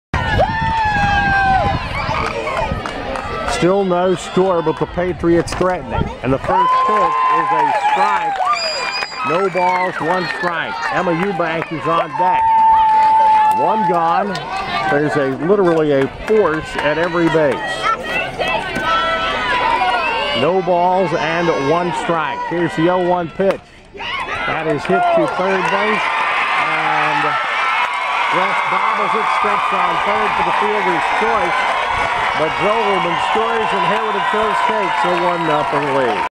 Regional Softball Heritage Hills 1 Jasper 0
The only run of the game was scored on a Stdney Richard FC scoring Mariah Zogleman. Emma Euban kpitched a two hit shutout as Heritage Hills edged Jasper 1-0